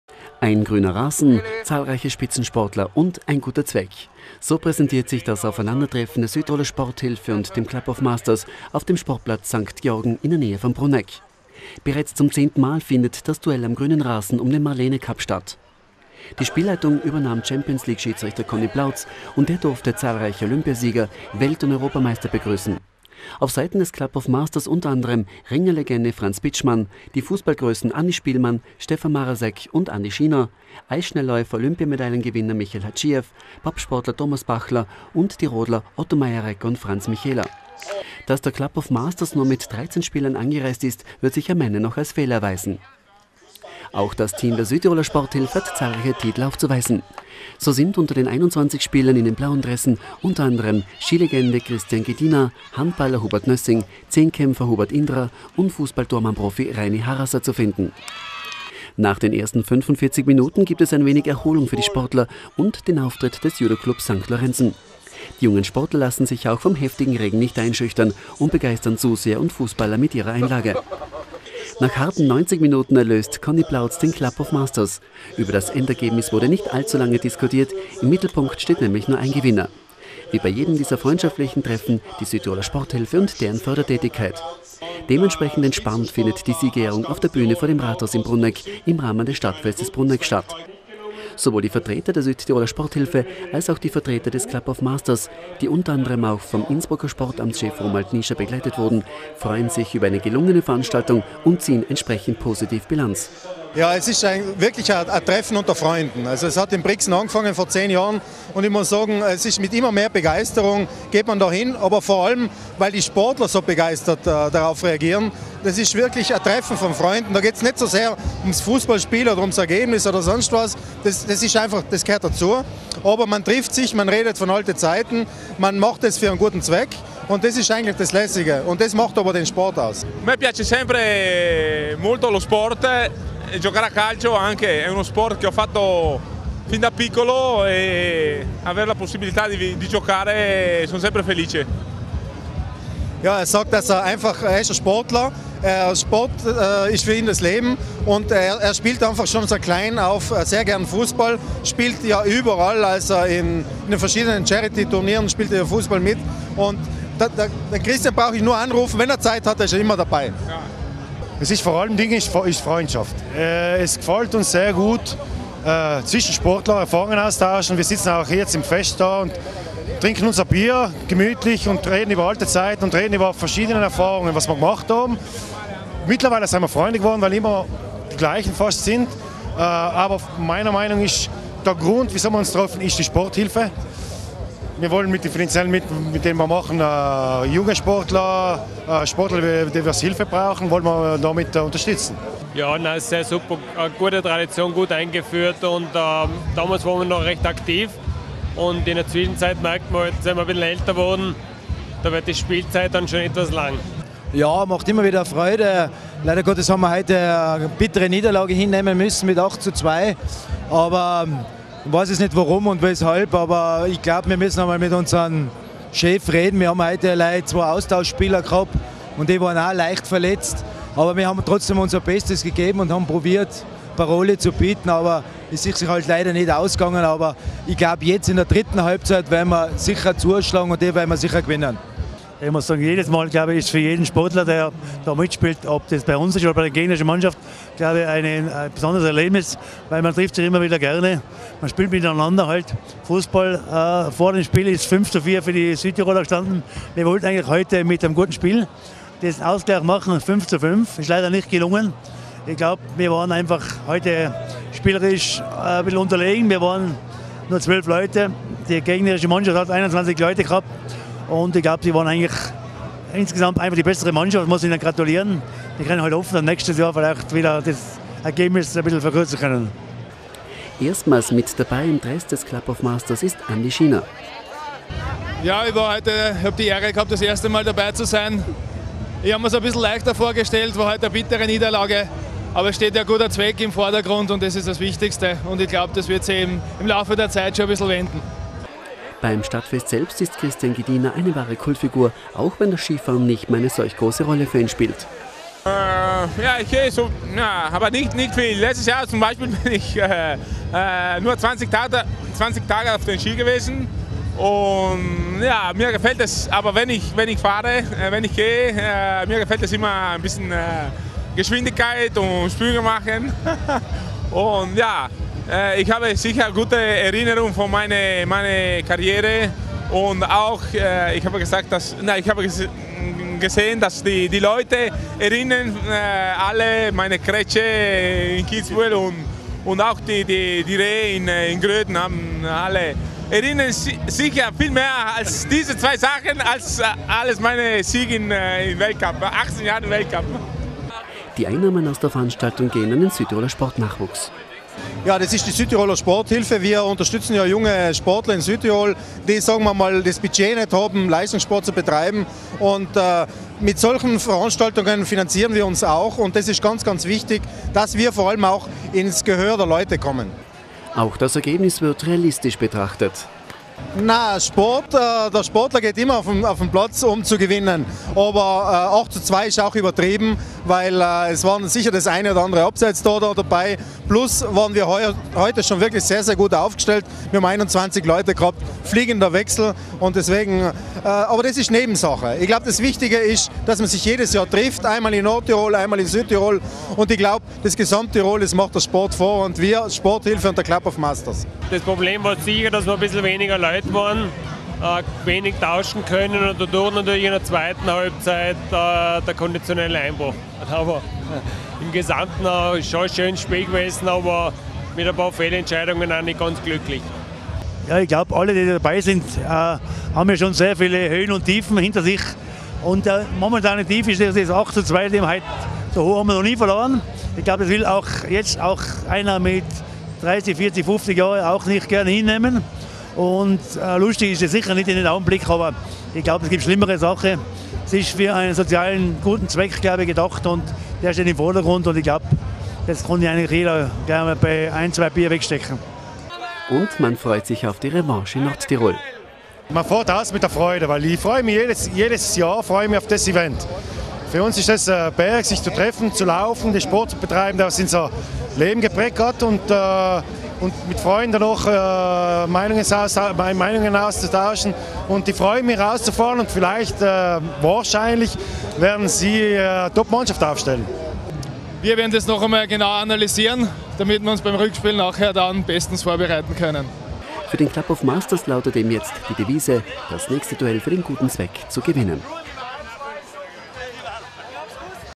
Club of Masters vs. Südtiroler Sporthilfe 2011;
In St. Georgen bei Bruneck kommt es zum legendären Duell zwischen der Südtiroler Sporthilfe und dem Club of Masters; das Duell am grünen Rasen für einen guten Zweck und anschließender Feier beim Stadtfest Bruneck; (C)2011